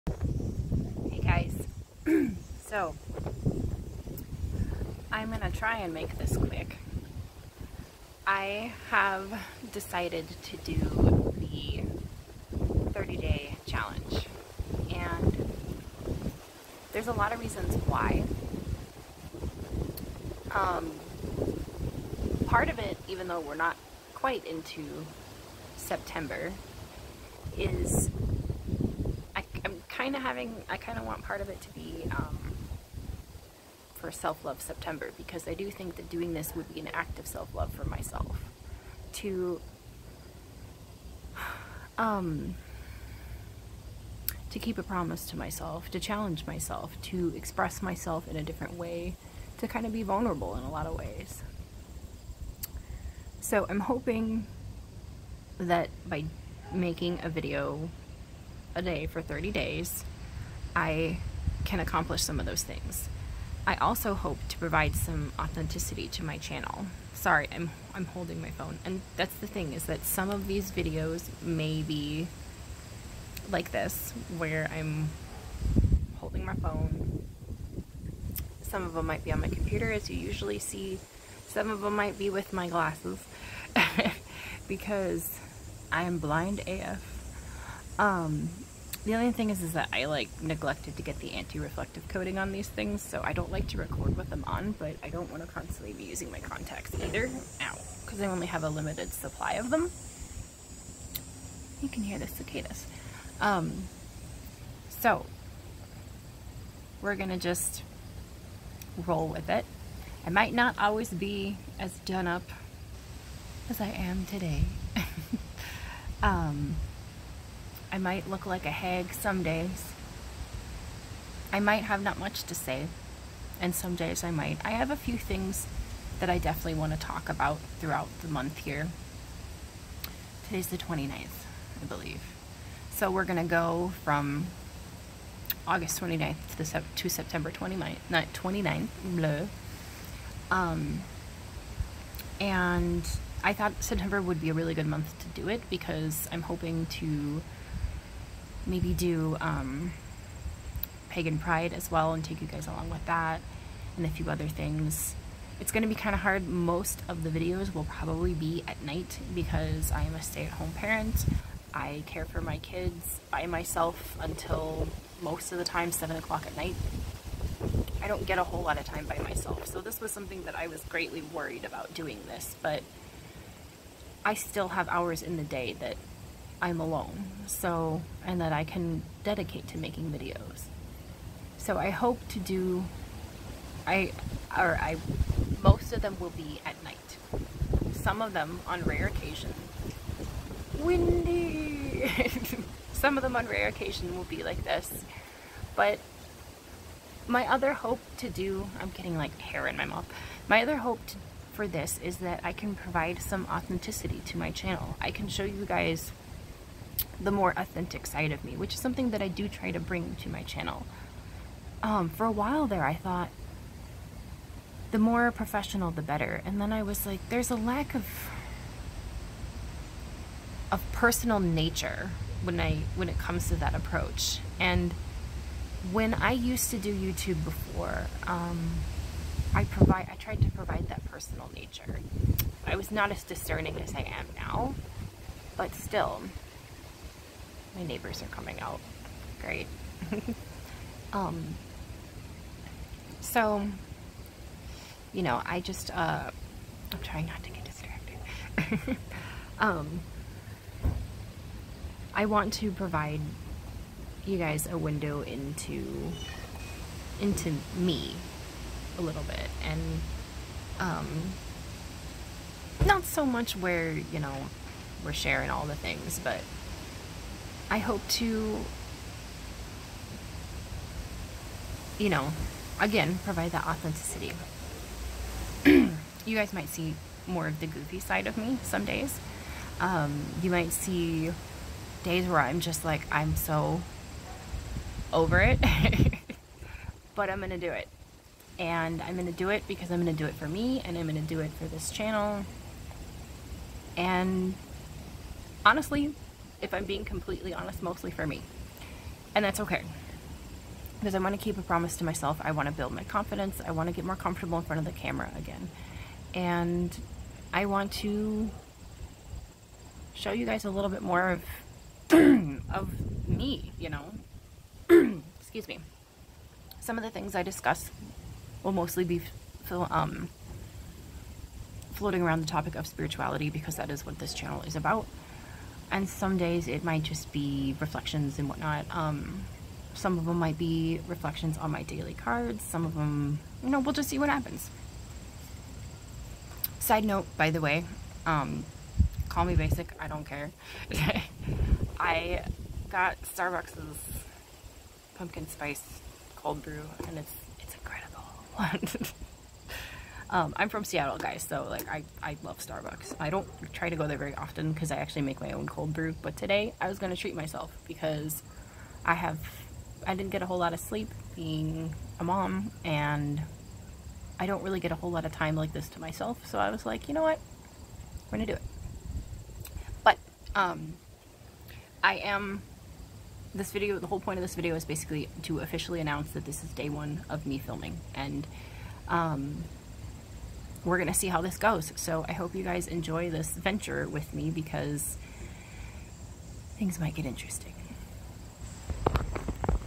Day 1 - Here we go
I'm officially starting the 30 filming challenge started by Imogen Walters. I will be filming a video once a day until September 29th. I hope you all enjoy this journey with me.